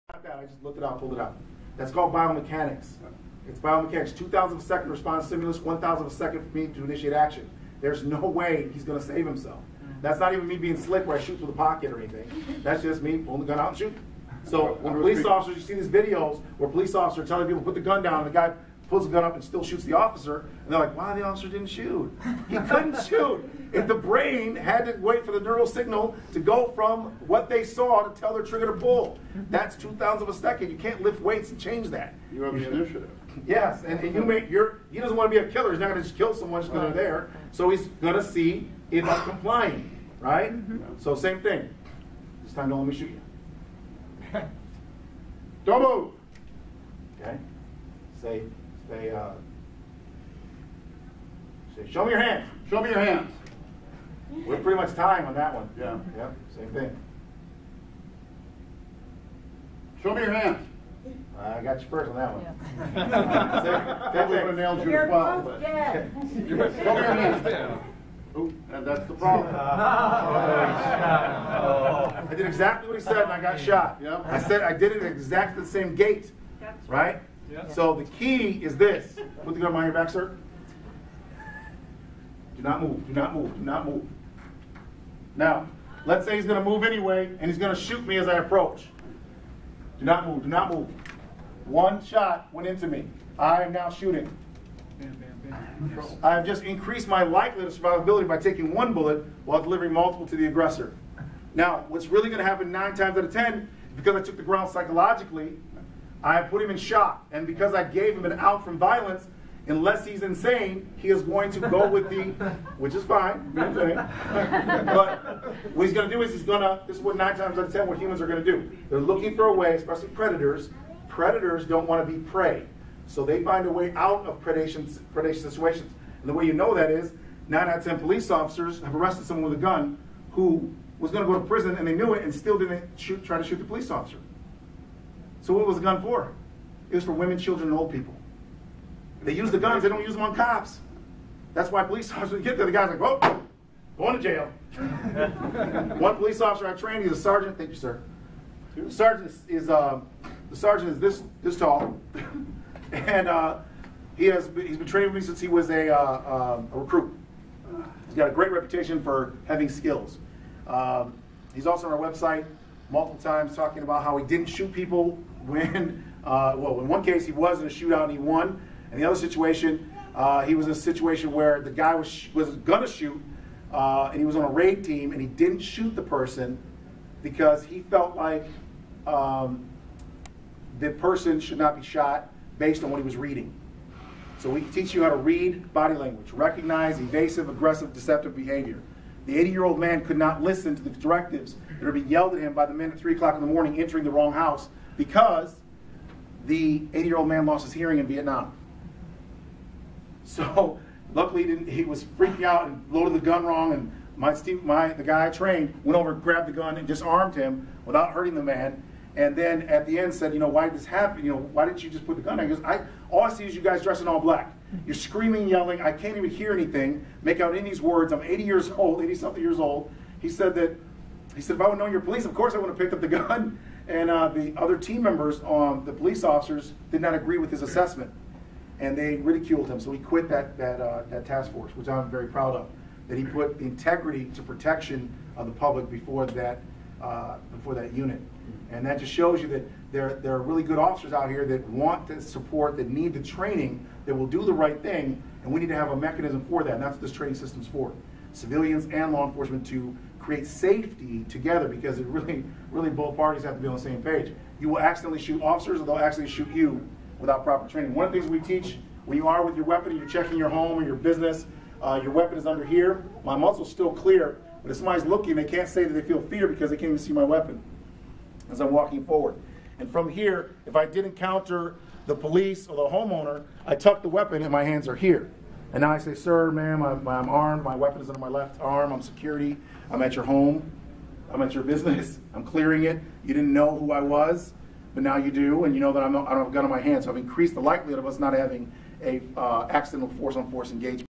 2017 02 04 Dale Brown - Reaction Time
Radisson Hotel - Manchester, NH
Day 3 of 3 during the 2017 New Hampshire Liberty Forum. Dale Brown of the Detroit Threat Management Center presents 'Preventative Threat Management - Nonviolent Public Safety'. Segment from the presentation featuring a volunteer from the audience.
Captured using a Nikon Coolpix P520